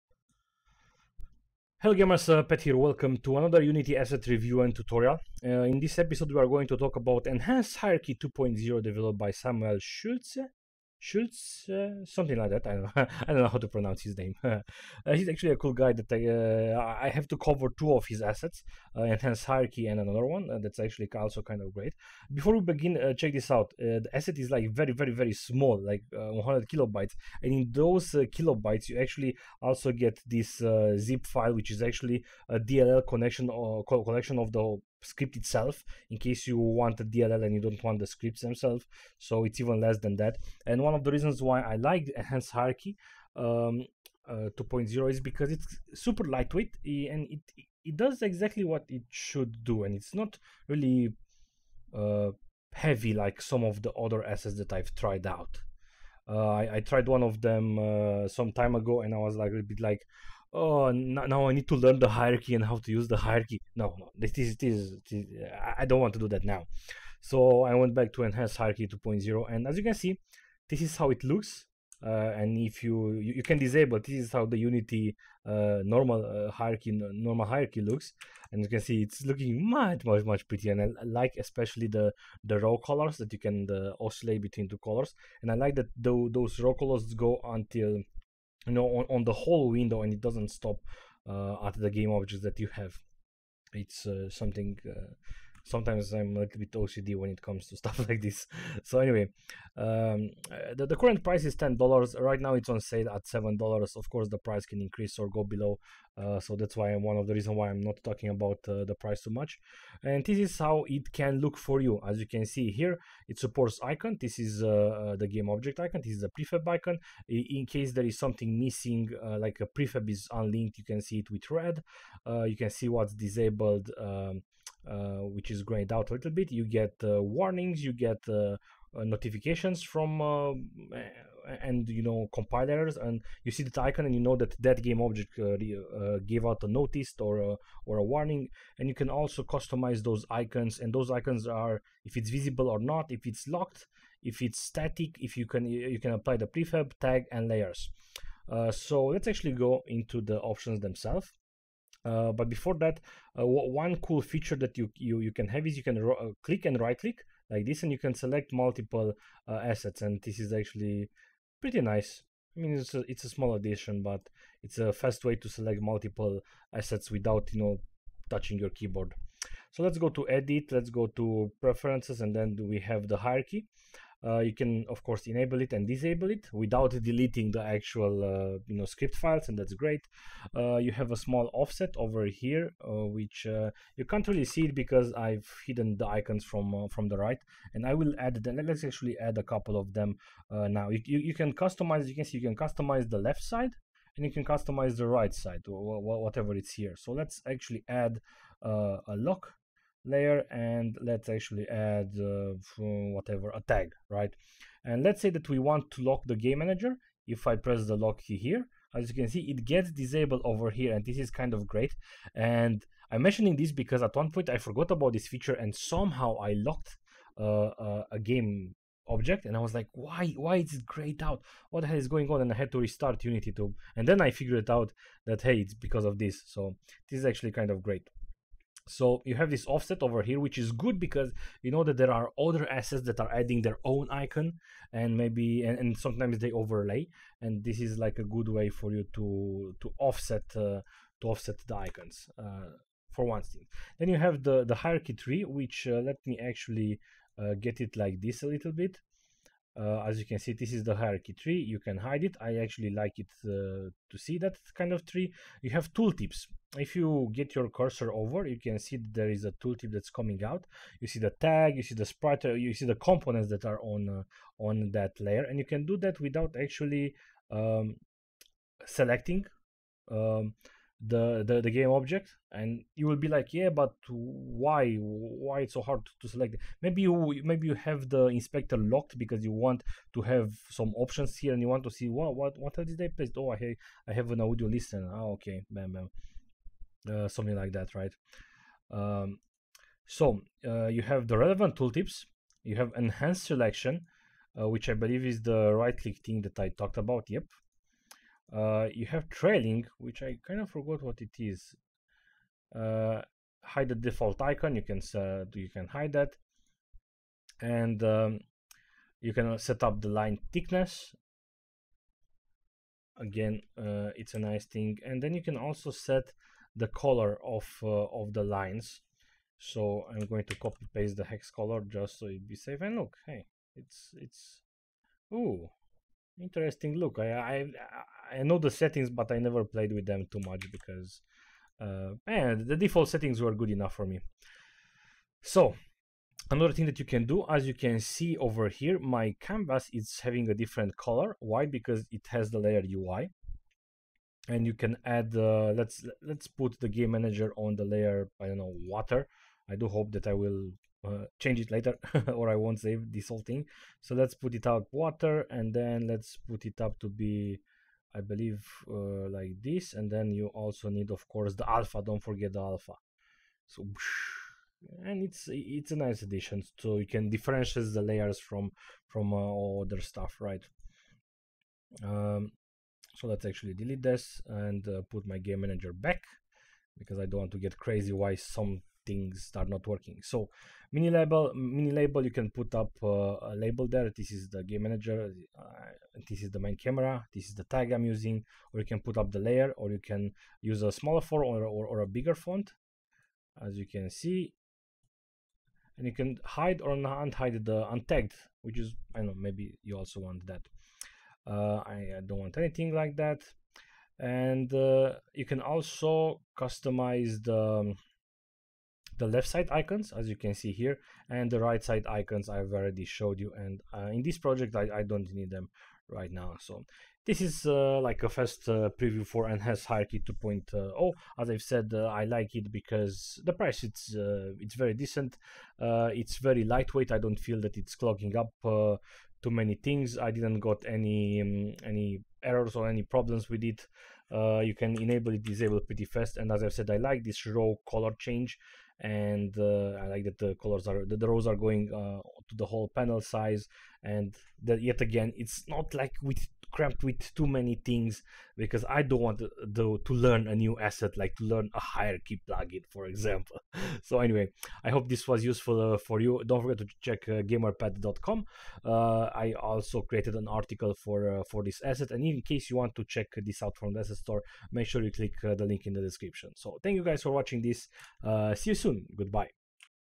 Enhanced Hierarchy 2.0 - Unity Asset Review & Tutorial
Enhanced Hierarchy 2.0 is an Unity an Unity Asset developed by Samuel Schultze. This is a lightweight Unity Asset that is going to enhance your hierarchy greatly improving your productivity!

One of the first Unity Assets that I install when I'm starting a new project.

Thanks for watchign!